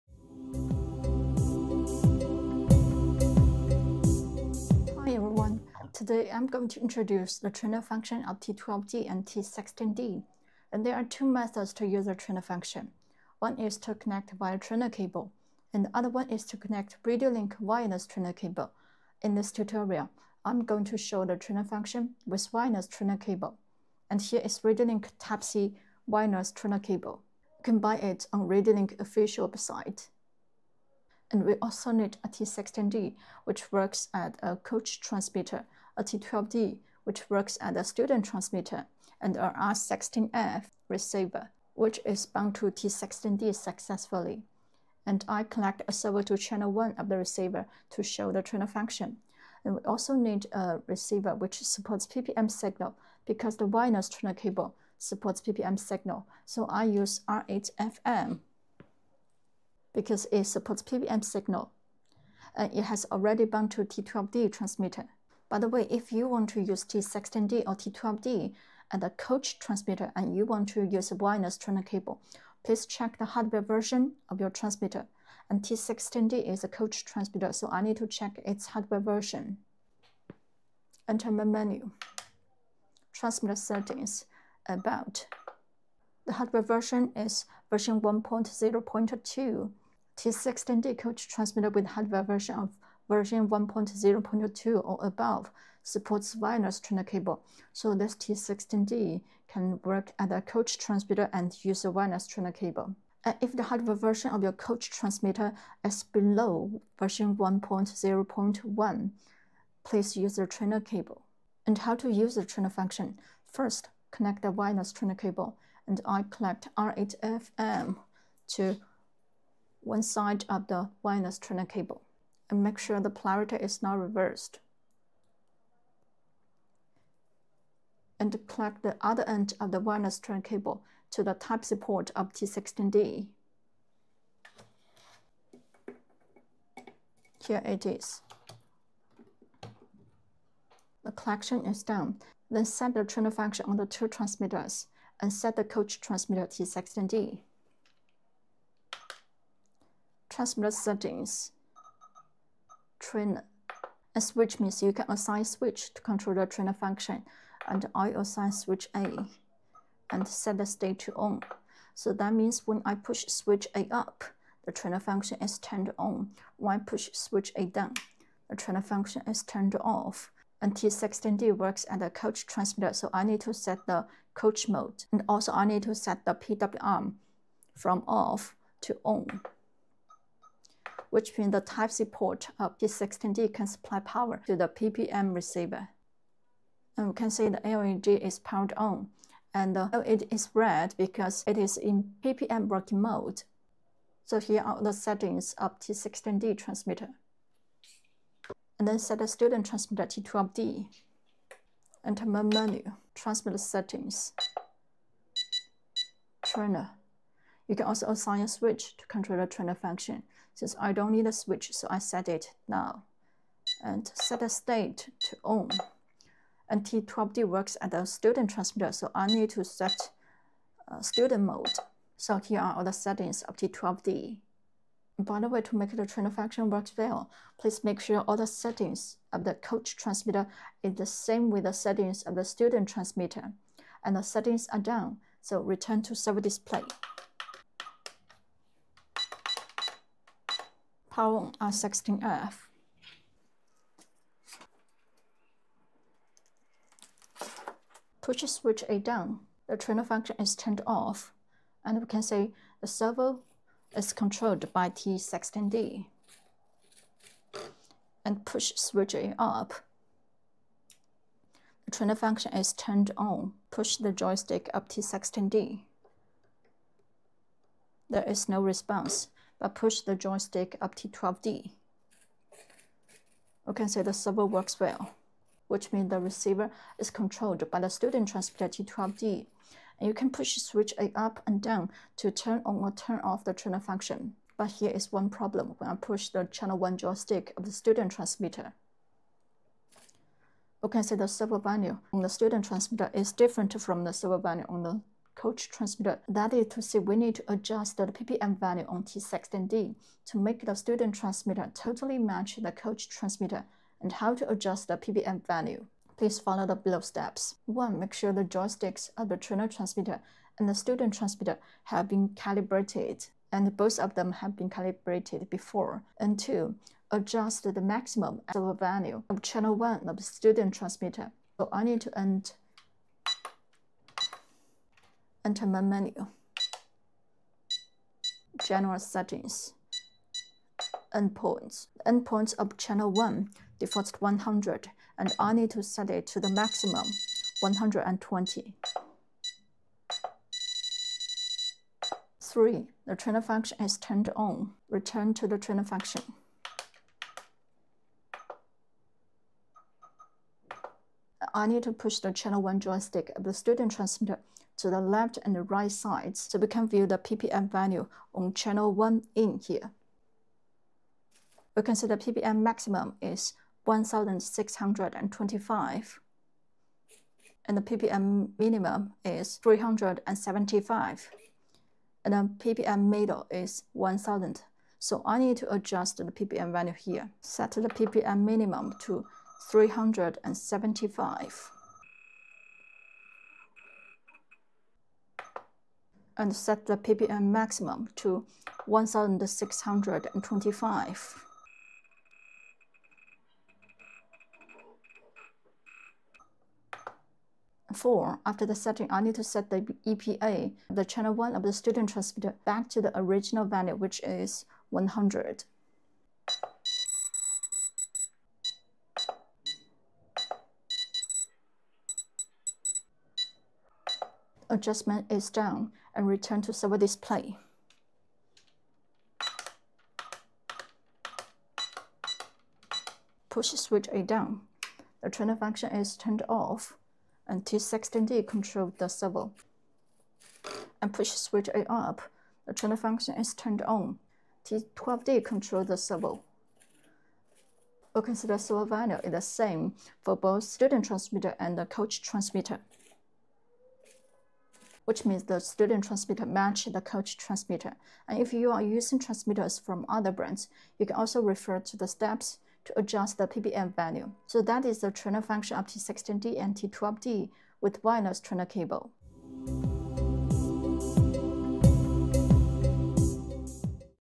T16D/T12D Trainer Function with Wireless Trainer Cable
Note: T16D, coach transmitter, with hardware version of V1.0.2 or above supports wireless trainer cable.
There are two methods to use the trainer function for T16D/T12D. One is to connect via trainer cable and the other is to connect via RadioLink wireless trainer cable.
In this tutorial, I’m going to show the trainer function with wireless trainer cable connection.
Here is RadioLink type-C wireless trainer cable we need.
We also need a T16D, which works as a coach transmitter, a T12D which works as a student transmitter, a R16F which is bound to T16D successfully. I connect a servo to the channel 1 of the receiver to better show the trainer function.
We also need a receiver which supports PPM signal, because the wireless trainer cable supports receivers of PPM signal. So I use R8FM mini receiver. This R8FM has already been bound to the student transmitter T12D.
By the way, if you want to use T16D/T12D as a coach transmitter and you want to use the wireless trainer cable, please check the hardware version of your T16D.
T16D, coach transmitter, with hardware version of V1.0.2 or above supports wireless trainer cable.
Enter MAIN MENU--Transmitter Settings--ABOUT. The hardware version is V1.0.2, so this T16D supports wireless trainer cable. If the hardware version of your coach transmitter T16D is below V1.0.2, please use the trainer cable.
How to use the trainer function?
First, connect the wireless trainer cable. I connect R8FM to the end of the trainer cable. And connect the trainer cable to the type-c port of T16D.
Then. Set trainer function on the two transmitters.
Set the coach transmitter T16D. Enter the MAIN MENU--Transmitter Settings--TRAINER. And I assign SWA_UP to control the trainer function, Set the state to ON. That means when SWA is pushed up, the trainer function is turned on; when SWA is pushed down, the trainer function is turned off.
T16D is the coach transmitter, so I set the mode to Coach mode.
And I need to set PWR to ON, which means the type-C port can supply power to the PPM receiver. And the LED of R8FM is powered on. We can see the LED of the receiver is in red, which means it is in PPM working mode.
Then. Set the student transmitter T12D. Enter the MAIN MENU--Transmitter Settings--TRAINER. I keep the switch NULL, because I don’t need switch to control it. You can assign a switch according to your needs. Set the state to ON. T12D is the student transmitter, so I set the mode to Student mode. Here are the settings of T12D.
By the way, to make the trainer function works well, please make sure all the settings of the coach transmitter is the same with the settings of the student transmitter.
The settings are done. Return to servo display. Push SWA down, the trainer function is turned off. The servo is connected to channel 1 of the receiver. So I push the joystick of T16D, the servo is controlled by the coach transmitter, T16D.
Push SWA up, the trainer function is turned on. Push the joystick of T16D, there is no response. Push the joystick of T12D, we can see the servo is working, which means the receiver is controlled by the student transmitter, T12D.
You can push use SWA to turn on or turn off the trainer function.
But here is one problem. When I push the channel 1 joystick of the student transmitter, the servo value on the student transmitter is different from the servo value on the coach transmitter. That is because we need to adjust the PPM value on T16D to make the student transmitter totally match the coach transmitter. How to adjust the PPM value here? Please follow the below steps.
1.Make sure the joysticks of the trainer and student transmitters have been correctly calibrated. Both of them have been calibrated before.
2.Adjust the maximum servo value of channel 1 of the student transmitter. Enter END POINT. The EPA of channel 1 of T8FB defaults to 100. We need to set it to the largest value 120.
3.The trainer function is turned on. Push the channel 1 joystick of the student transmitter to the left and right sides. We can view the PPM value of CH1 IN of the student transmitter on T16D. We can see the PPM maximum is 1625, PPM minimum is 375, PPM middle is about 1000. So we need to adjust PPM MIN to 375, set PPM MAX to 1625, and PPM MID to 1000.
4.After the setting, set the EPA of the channel 1 of the student transmitter back to the original value. It defaults to 100. So I set it back to 100.
The adjustment of PPM value is done. Return to servo display. Push down, the trainer function is turned off. T16D controls the servo. Here is the movement of the servo.
If you are using transmitters from other brands, you can also refer to the steps to adjust the PPM value.
So that is the trainer function of T16D and T12D with Wireless Trainer Cable.